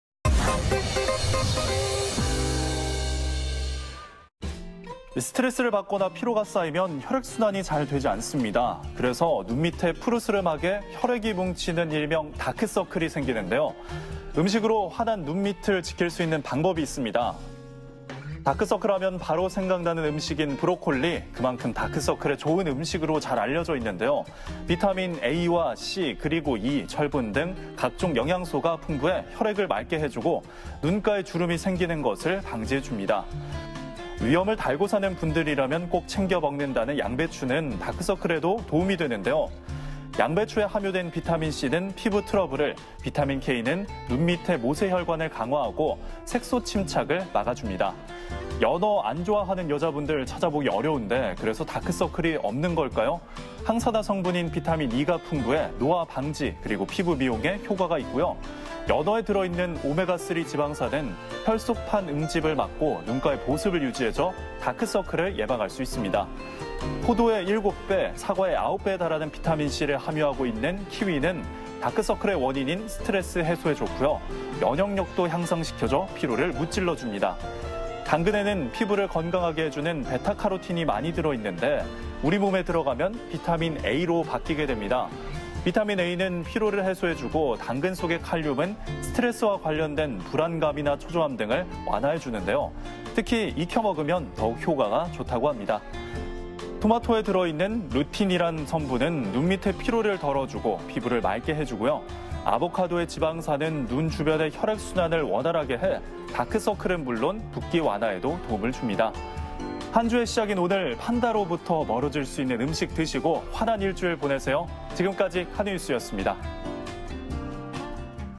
[카드뉴스] 눈 밑 그림자, 00 먹고 지워 버리세요~!
스트레스를 받거나 피로가 쌓이면 혈액순환이 잘 되지 않습니다.

그래서 눈 밑에 푸르스름하게 혈액이 뭉치는 일명 다크서클이 생기는데요.

음식으로 환한 눈 밑을 지킬 수 있는 방법이 있습니다.

다크서클하면 바로 생각나는 음식인 브로콜리.

그만큼 다크서클에 좋은 음식으로 잘 알려져 있는데요.

비타민 A와 C 그리고 E, 철분 등 각종 영양소가 풍부해 혈액을 맑게 해주고, 눈가에 주름이 생기는 것을 방지해줍니다.

위염을 달고 사는 분들이라면 꼭 챙겨 먹는다는 양배추는 다크서클에도 도움이 되는데요.

양배추에 함유된 비타민C는 피부 트러블을, 비타민 K는 눈 밑의 모세혈관을 강화하고 색소 침착을 막아줍니다.

연어 안 좋아하는 여자분들 찾아보기 어려운데, 그래서 다크서클이 없는 걸까요?

항산화성분인 비타민 E가 풍부해 노화 방지 그리고 피부미용에 효과가 있고요.

연어에 들어있는 오메가3 지방산은 혈소판 응집을 막고 눈가의 보습을 유지해줘 다크서클을 예방할 수 있습니다.

포도의 7배, 사과의 9배에 달하는 비타민 C를 함유하고 있는 키위는 다크서클의 원인인 스트레스 해소에 좋고요.

면역력도 향상시켜줘 피로를 무찔러 줍니다.

당근에는 피부를 건강하게 해주는 베타카로틴이 많이 들어있는데, 우리 몸에 들어가면 비타민 A로 바뀌게 됩니다.

비타민 A는 피로를 해소해주고, 당근 속의 칼륨은 스트레스와 관련된 불안감이나 초조함 등을 완화해주는데요.

특히 익혀 먹으면 더욱 효과가 좋다고 합니다.

토마토에 들어있는 루틴이란 성분은 눈의 피로를 덜어주고 피부를 맑게 해주고요.

아보카도의 지방산은 눈 주변의 혈액순환을 원활하게 해 다크서클은 물론 붓기 완화에도 도움을 줍니다.

한 주의 시작인 오늘, 판다로부터 멀어질 수 있는 음식 드시고 환한 일주일 보내세요!